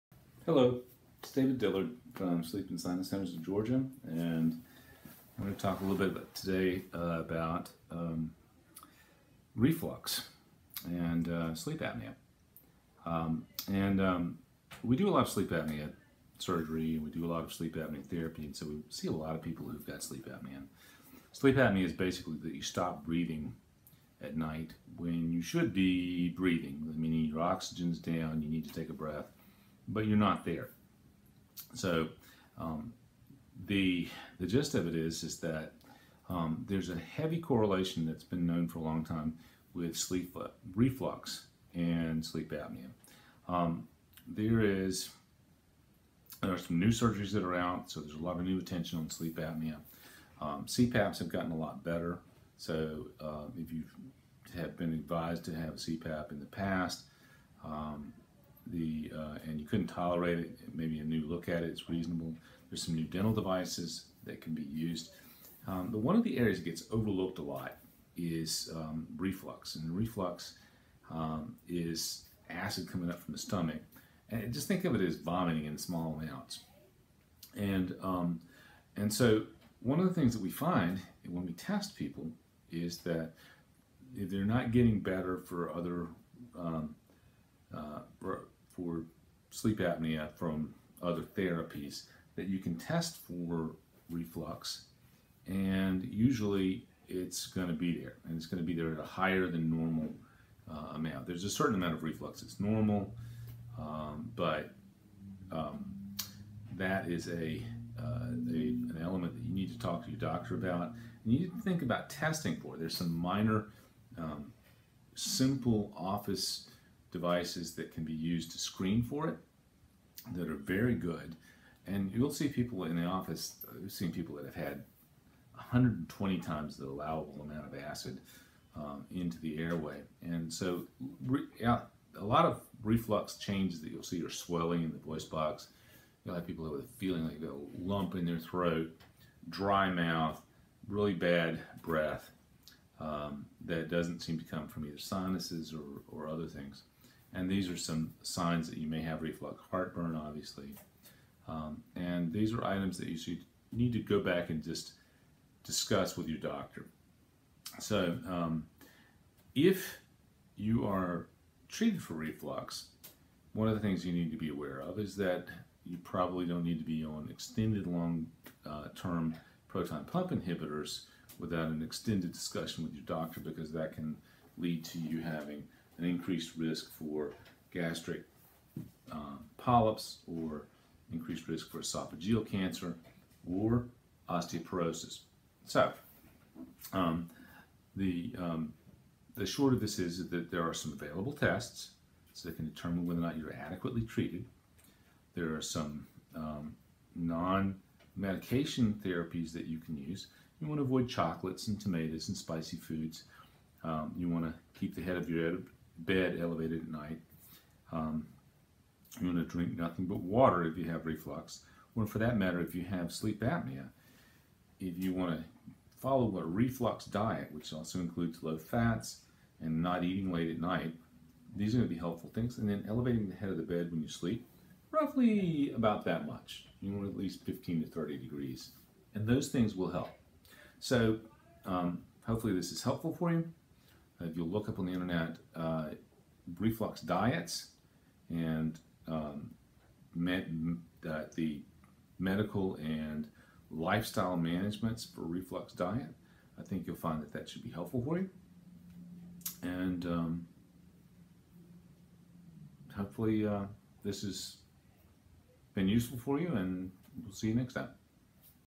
Reflux and Sleep Apnea
There is a heavy correlation between reflux and Sleep Apnea. Reflux is acid that comes up from the stomach, like vomiting in small amounts. There are some minor office devices that can screen for reflux. There is a normal amount of reflux that most people have, but if you have an access then you need to see a doctor. There are some non medication treatments you can use such as eating spicy foods, sleeping with your head lifted at night and certain diets.